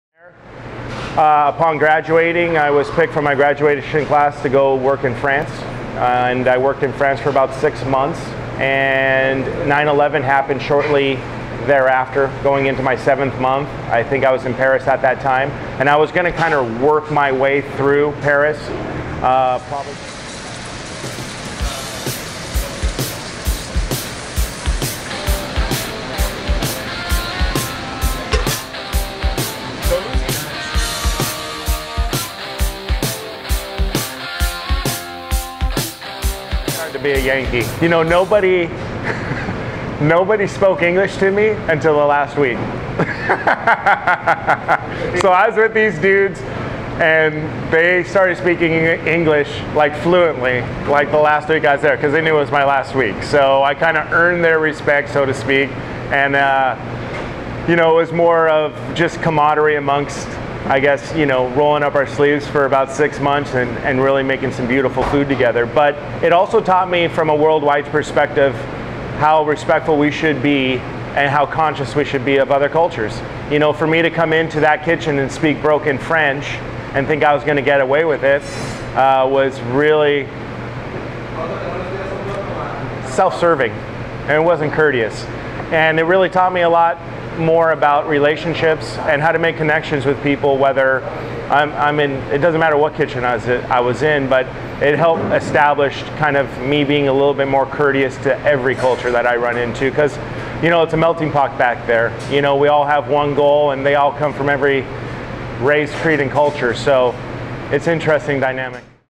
Great Kitchen Pranks
Funny story about a kitchen prank that happened to Joseph in France.